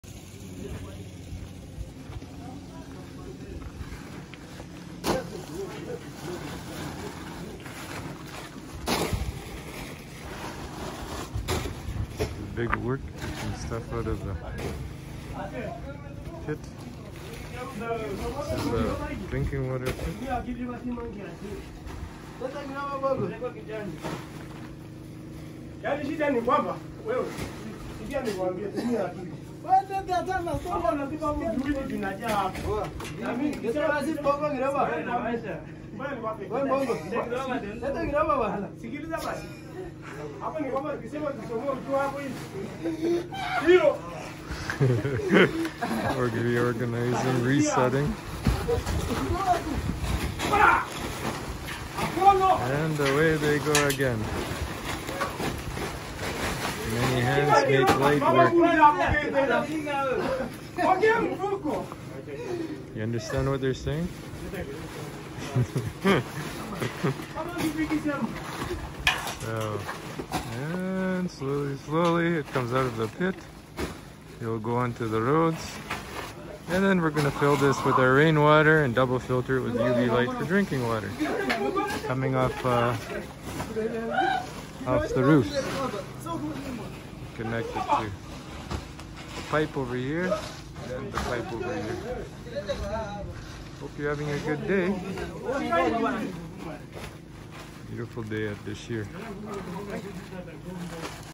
Removing Soil for the Pit
Our neighbor guys take out soil from a pit. Digging with sledge hammers and big chisels, we are now 5 metres below ground - space to hold about 30,000 litres of rain water. We will run the water through two filters and UV light to create crystal clean Dashir drinking water for our workers and guests. Almost there!